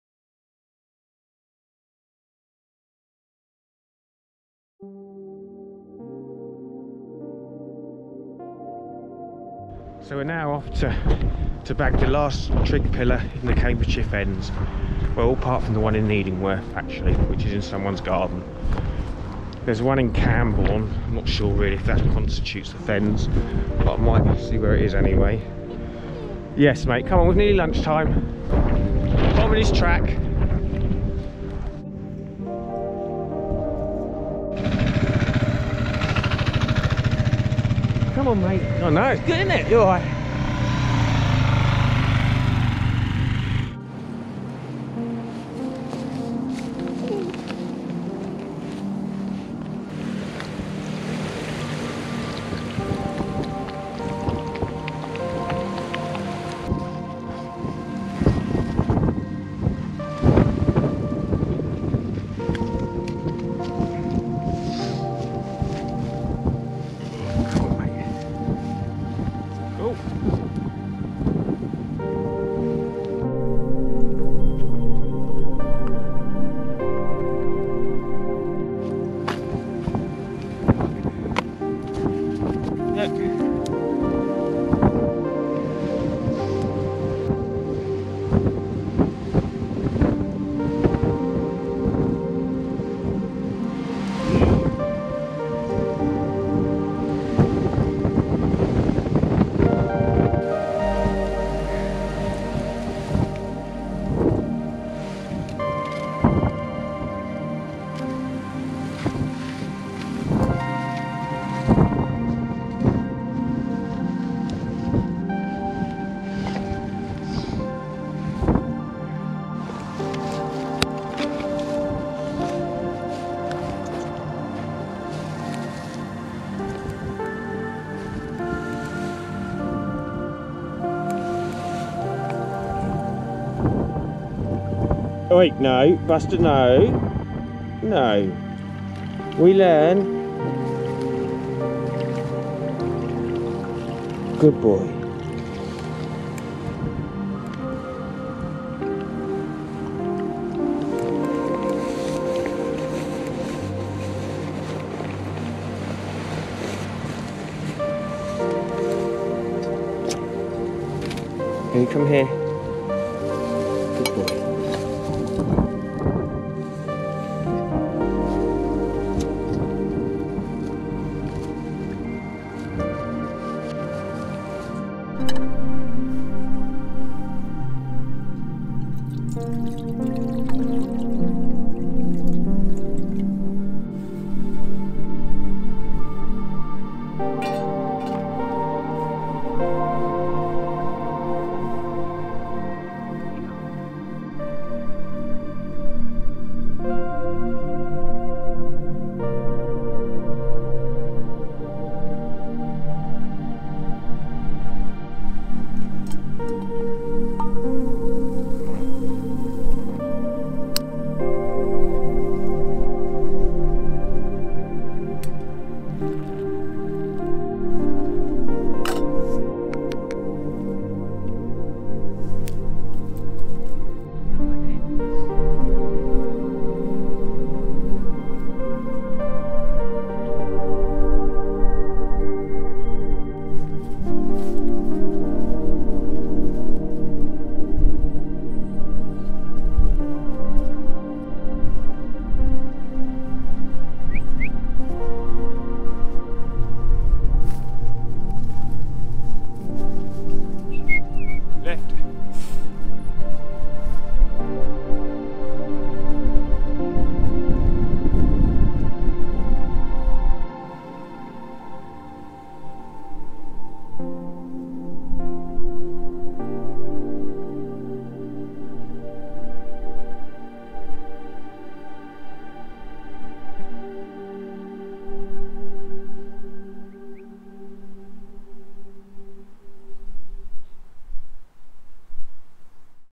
Street Way Trig Point
The last but one Trig Pillar in the Cambridgeshire Fens.

Buster enjoying Street Way Ordnance Survey Trig Point (near Great Wilbraham / Six Mile Bottom). Flush Bracket 32.70m above Ordnance Datum Newlyn, last verified in 1956.

Grid Reference:  TL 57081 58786

Must be time for another episode soon

As always.. thanks for stopping by.